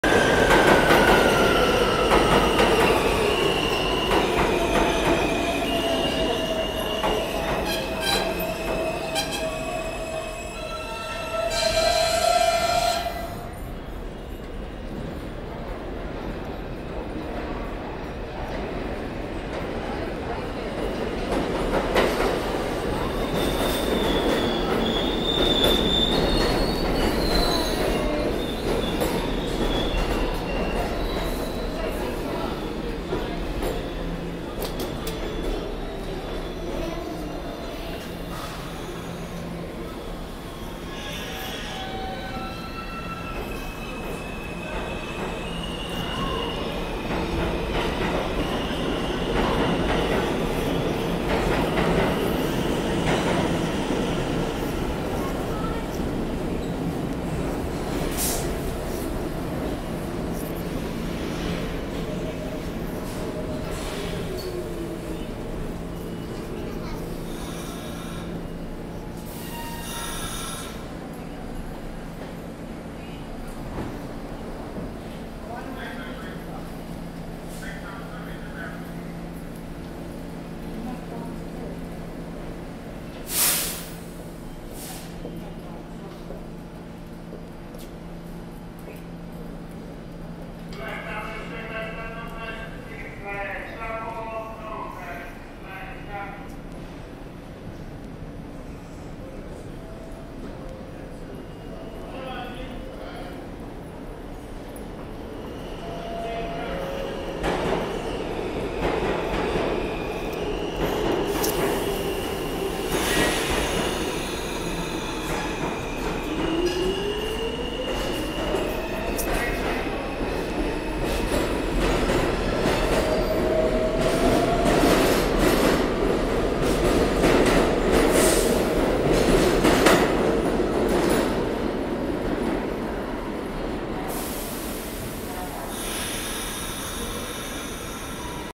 179 St bound R46 F at Forest Hills
Only 2 of these ran on the F today...i think R46s will be gone from the F permanently very soon.